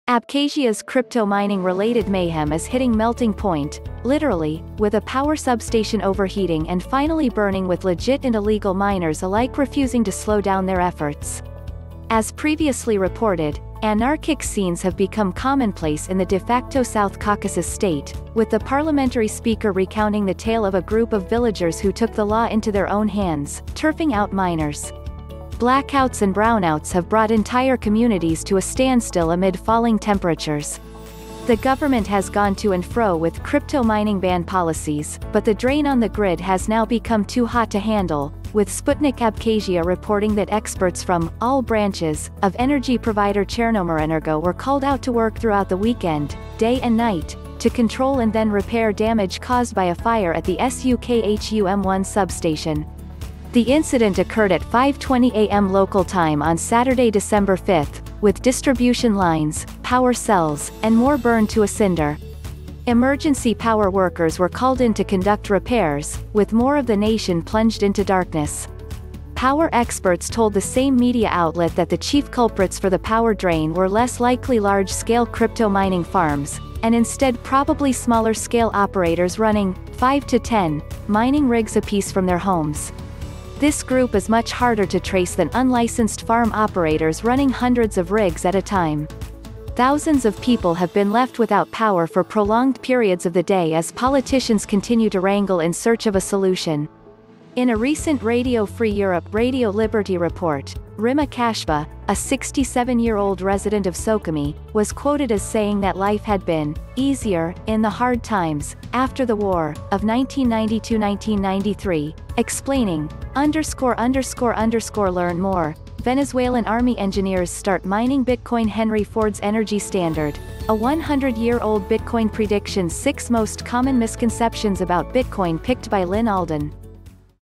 Power Substation Burns out as Abkhazia Bitcoin Mining Woes Worsen
Abkhazia’s crypto mining-related mayhem is hitting melting point – literally – with a power substation overheating and finally burning with legit and illegal miners alike refusing to slow down their efforts. As previously reported, anarchic scenes have become commonplace in the de facto South Caucasus state, with the parliamentary speaker recounting the tale of a group of villagers who took the law into their own hands, turfing out miners. Blackouts and brownouts have brought entire communities to a standstill amid falling temperatures. The government has gone to and fro with crypto mining ban policies, but the drain on the grid has now become too hot to handle – with Sputnik Abkhazia reporting that experts from “all branches” of energy provider Chernomorenergo were called out to work throughout the weekend, day and night, to control and then repair damage caused by a fire at the Sukhum-1 substation. The incident occurred at 5:20 am local time on Saturday, December 5, with distribution lines, power cells, and more burned to a cinder. Emergency power workers were called in to conduct repairs, with more of the nation plunged into darkness. Power experts told the same media outlet that the chief culprits for the power drain were less likely large-scale crypto mining farms – and instead probably smaller-scale operators running “five to ten” mining rigs apiece from their homes. This group is much harder to trace than unlicensed farm operators running hundreds of rigs at a time. Thousands of people have been left without power for prolonged periods of the day as politicians continue to wrangle in search of a solution. In a recent Radio Free Europe/Radio Liberty report, Rimma Khashba, a 67-year-old resident of Sukhumi, was quoted as saying that life had been “easier” in the hard times “after the war” of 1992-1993, explaining,___Learn more:Venezuelan Army Engineers Start Mining BitcoinHenry Ford’s Energy Standard: A 100-Year Old Bitcoin Prediction6 Most Common Misconceptions About Bitcoin Picked by Lyn Alden